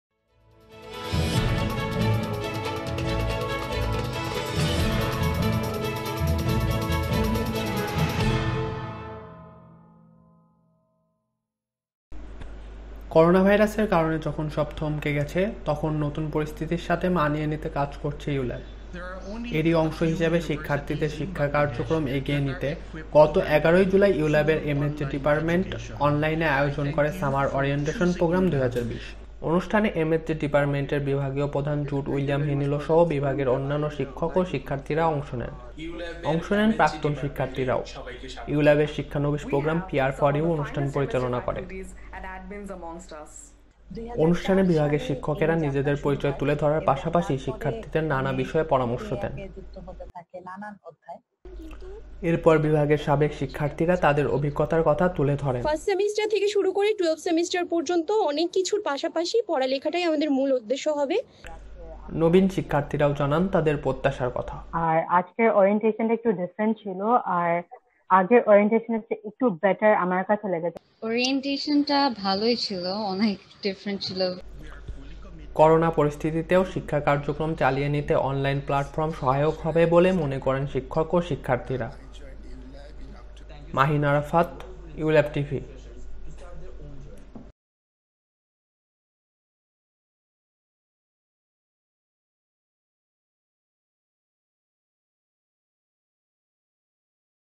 শিক্ষার্থীদের শিক্ষা কার্যক্রম এগিয়ে নিতে অনলাইনে সামার ওরিয়েন্টেশন প্রোগ্রাম ২০২০ এর আয়োজন
করোনা পরিস্থিতির সঙ্গে খাপ খাইয়ে নিতে ও শিক্ষা কার্যক্রম এগিয়ে নিতে, ইউল্যাবের এম এস জে ডিপার্টমেন্ট,  অনলাইনে আয়োজন করে সামার ওরিয়েন্টেশন প্রোগ্রাম ২০২০। এতে উপস্থিত ছিলেন শিক্ষক ও শিক্ষার্থীরা। রেদওয়ান আহম্মেদের তথ্যে বিস্তারিত মাহিন আরাফাতের প্রতিবেদনে।